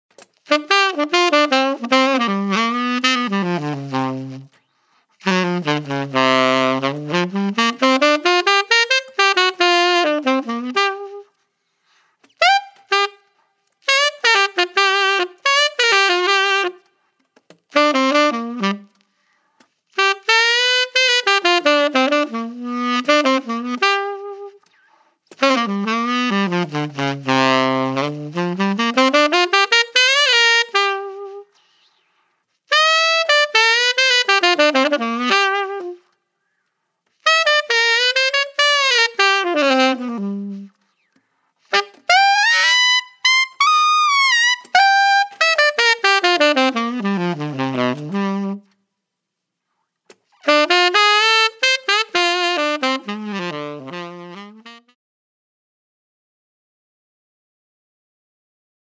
Drake Mouthpieces Ceramic Resonance Chamber Contemporary Tenor Model
Hello Friends,  Here is a sample of the revolutionary Ceramic Resonance Chamber Contemporary Tenor Model.
As the leading innovator in the field of ceramic acoustics, Drake has created a revolutionary manufacturing process which encapsulates the ceramic chamber in our vintage resin material.
The first major innovation in mouthpieces in over 50 years!
How does a ceramic chamber affect the sound?
The ceramic chamber contributes a new tone color to your sound.  The exposed ceramic on the interior of the mouthpiece contributes unique density properties and produces a strong core fundamental tone color, with a deep richness in the vibration of the air column.
These high baffle mouthpieces are designed for players looking for the ability to cut through and be heard, while still maintaining a balance overtone series. This model features a high baffle with a graduated chamber transition and large chamber. Great for R&B, Rock and Roll, and Contemporary Jazz Fusion.
– Includes Drake Double Rail Ligature, Drake branded cap, velvet pouch and storage case
Each mouthpiece is handmade to order by Aaron Drake.